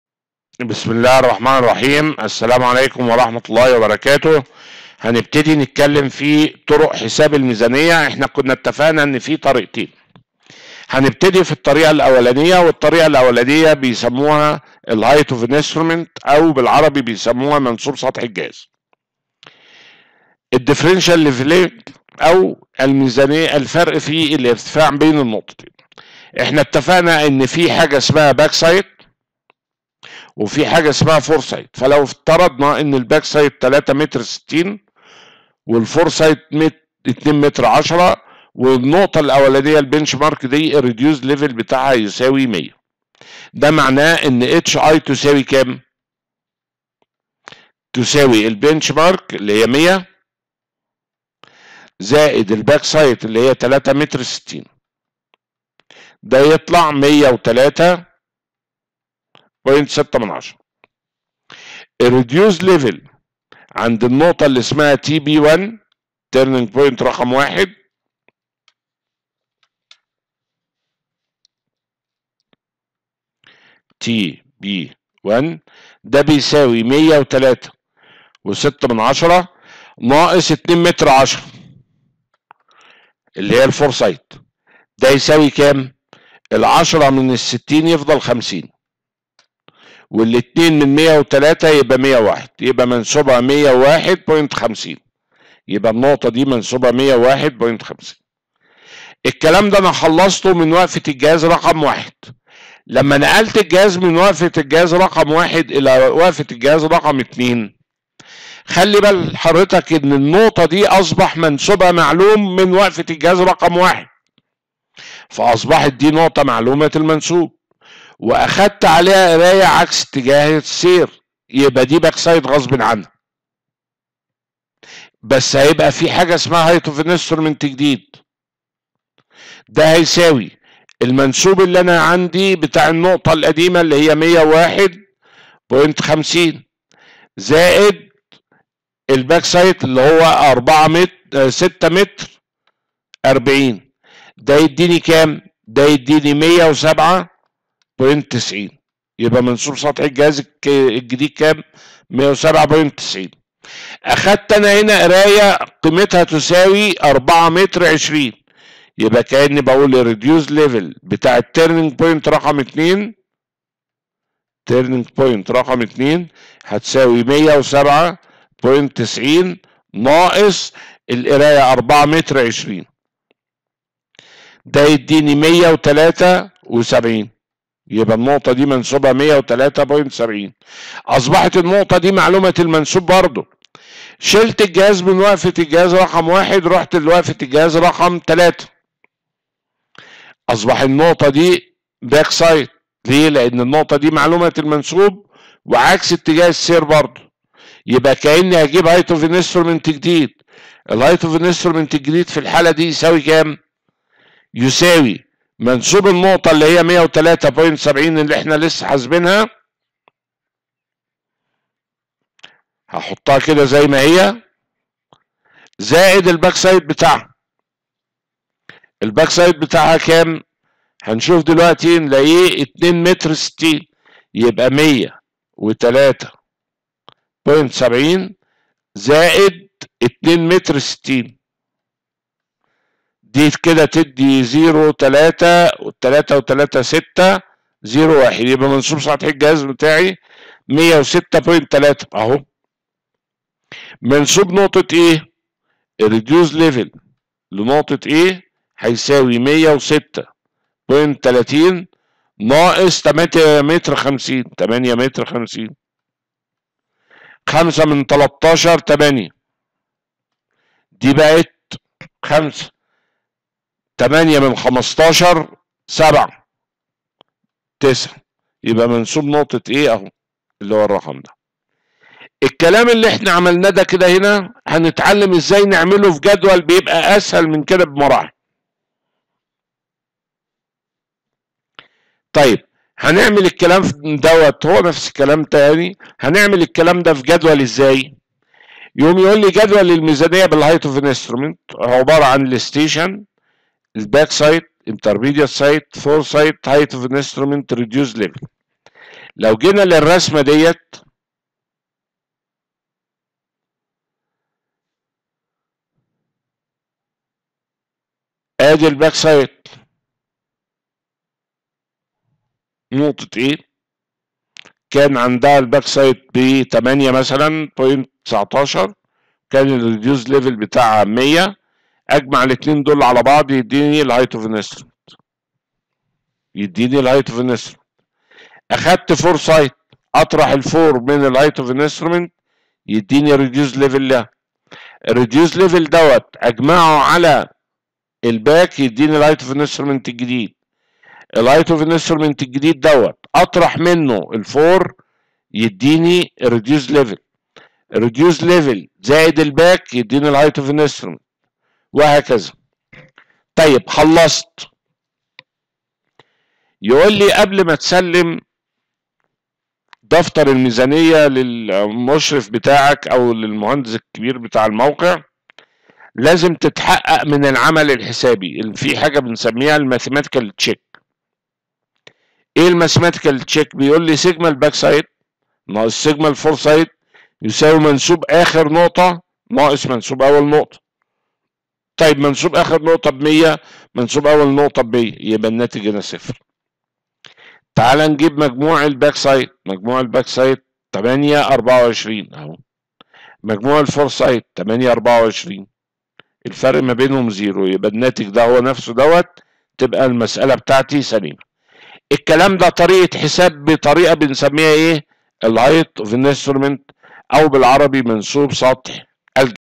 Surveying | 8-4 | Differential Leveling
المقررات المفتوحة - مقرر المساحة  - Surveying
Chapter 8 : LEVELING—THEORY AND METHODS
Lecture 4 : Differential Leveling
• Differential Leveling
• HEIGHT OF INSTRUMENT